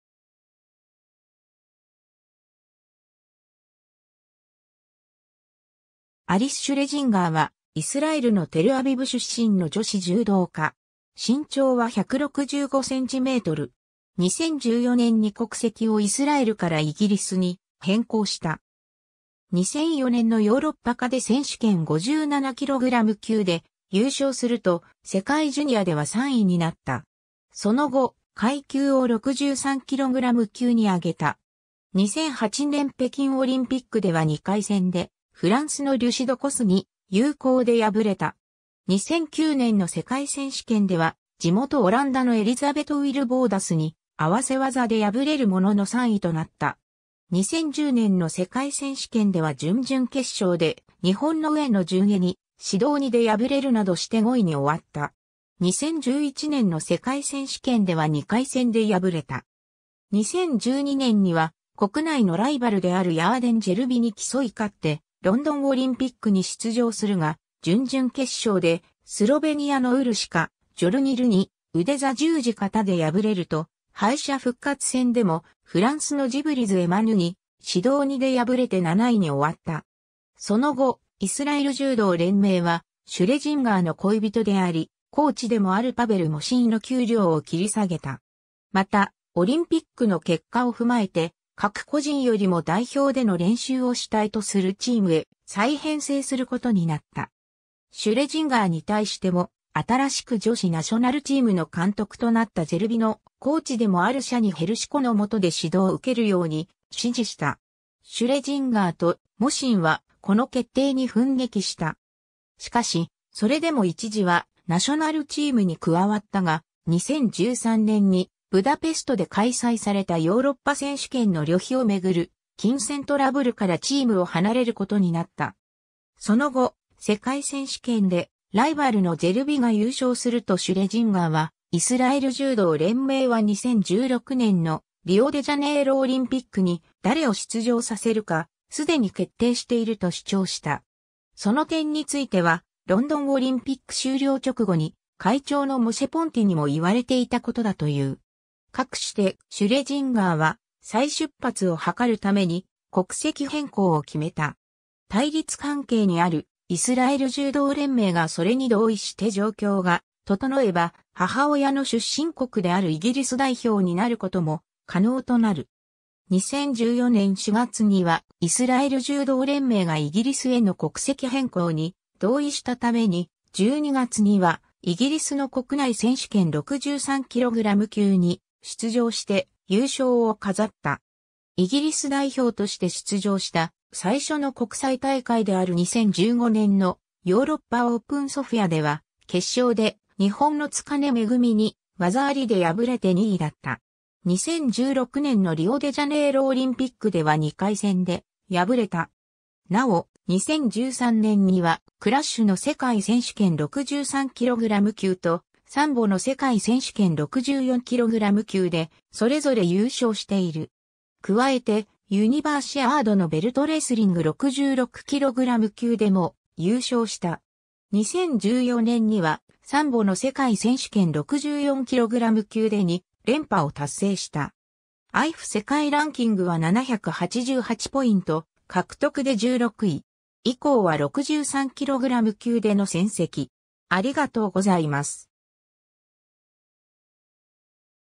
アリス・シュレジンガー
アリス・シュレジンガー（ヘブライ語: אליס שלזינגר‎ 、英: Alice Hester Elizabeth Schlesinger、1988年5月26日 - ）はイスラエルのテルアビブ出身の女子柔道家。
身長は165cm。
2014年に国籍をイスラエルからイギリスに変更した。
 2004年のヨーロッパカデ選手権57kg級で優勝すると、世界ジュニアでは3位になった。
その後、階級を63kg級に上げた。
2008年北京オリンピックでは2回戦でフランスのリュシ・ドコスに有効で敗れた。
2009年の世界選手権では地元オランダのエリザベト・ウィルボーダスに合技で敗れるものの3位となった。
2010年の世界選手権では準々決勝で日本の上野順恵に指導2で敗れるなどして5位に終わった。
2011年の世界選手権では2回戦で敗れた。
2012年には国内のライバルであるヤーデン・ジェルビに競い勝ってロンドンオリンピックに出場するが、準々決勝でスロベニアのウルシカ・ジョルニルに腕挫十字固で敗れると、敗者復活戦でもフランスのジブリズ・エマヌに指導2で敗れて7位に終わった。
その後、イスラエル柔道連盟はシュレジンガーの恋人でありコーチでもあるパベル・モシンの給料を切り下げた。
また、オリンピックの結果を踏まえて、各個人よりも代表での練習を主体とするチームへ再編成することになった。
シュレジンガーに対しても、新しく女子ナショナルチームの監督となったジェルビのコーチでもあるシャニ・ヘルシュコの下で指導を受けるように指示した。
シュレジンガーとモシンはこの決定に憤激した。
しかし、それでも一時はナショナルチームに加わったが、2013年にブダペストで開催されたヨーロッパ選手権の旅費を巡る金銭トラブルからチームを離れることになった。
その後、世界選手権でライバルのジェルビが優勝するとシュレジンガーは、｢イスラエル柔道連盟は2016年のリオデジャネイロオリンピックに誰を出場させるか既に決定している｣と主張した。
その点についてはロンドンオリンピック終了直後に会長のモシェ・ポンティにも言われていたことだという。
かくして、シュレジンガーは再出発を図るために国籍変更を決めた。
対立関係にあるイスラエル柔道連盟がそれに同意して状況が整えば、母親の出身国であるイギリス代表になることも可能となる。
2014年4月にはイスラエル柔道連盟がイギリスへの国籍変更に同意したために、12月にはイギリスの国内選手権63kg級に出場して優勝を飾った。
イギリス代表として出場した最初の国際大会である2015年のヨーロッパオープン・ソフィアでは、決勝で日本の津金恵に技ありで敗れて2位だった。
 2016年のリオデジャネイロオリンピックでは2回戦で敗れた。
 なお、2013年にはクラッシュの世界選手権63kg級とサンボの世界選手権64kg級でそれぞれ優勝している。
加えて、ユニバーシアードのベルトレスリング66kg級でも優勝した。
2014年にはサンボの世界選手権64kg級で2連覇を達成した。
 IJF世界ランキングは788ポイント獲得で16位(19/4/22現在)。
 以降は63kg級での戦績 (出典、JudoInside.com)。